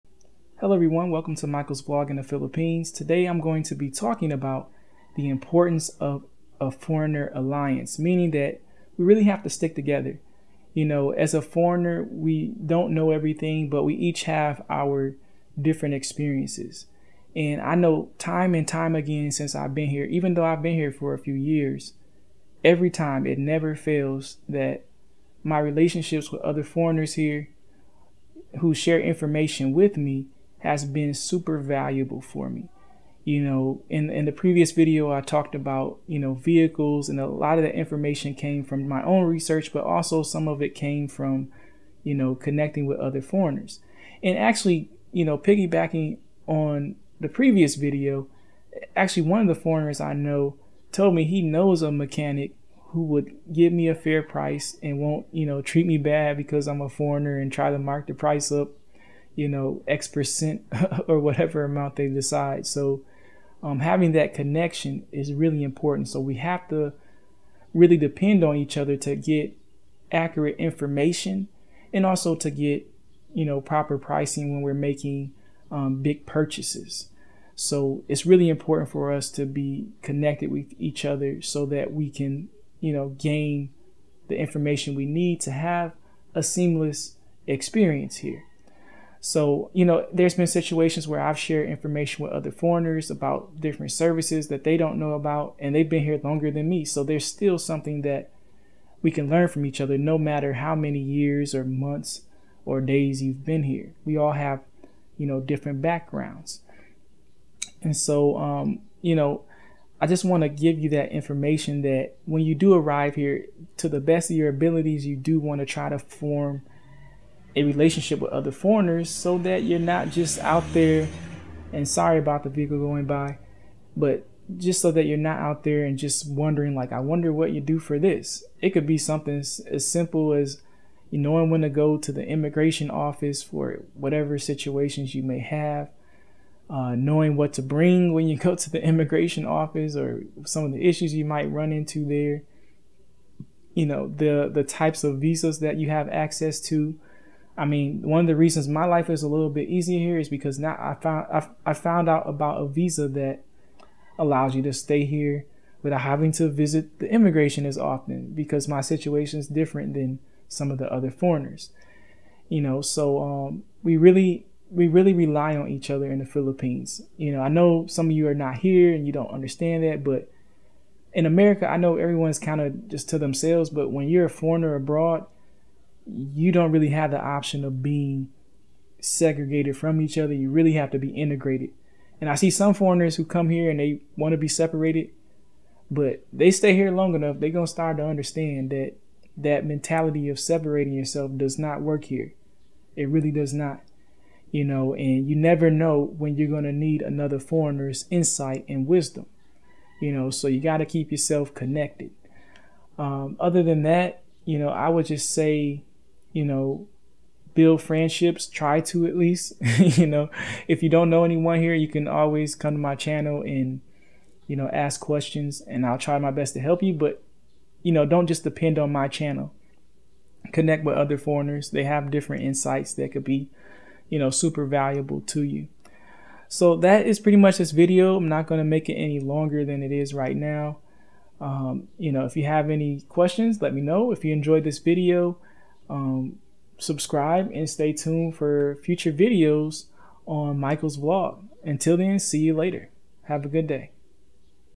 Importance of Foreigner Alliance In The Philippines
In my opinion it is essential to connect with other foreigners when you live in the Philippines so that you can gain insight into certain subject areas. This could range from many topics such as immigration, travel, purchasing products and so on. Davao Philippines is a very nice place but you must understand your environment as best as possible to flourish here.

I hope you enjoyed watching my vlog in the Philippines.